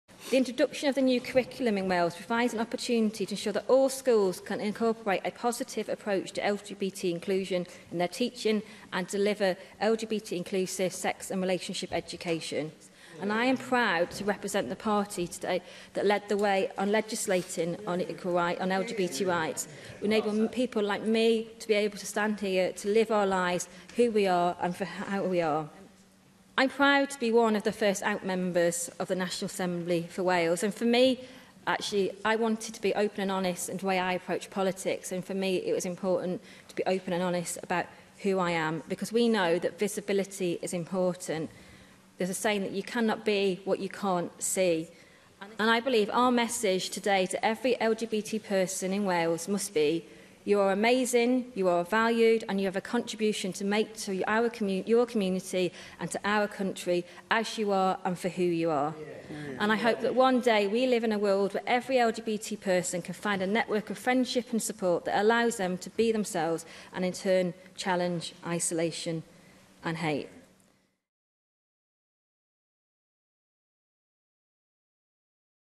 Why inclusive education matters
We've come a long way since Section28 and I'm pleased the Welsh Government is developing inclusive Relationships and Sexuality Education (RSE) as part of Wales’ new curriculum which will be in place from 2022.
Here's my speech in the Senedd during last year's LGBT History Month.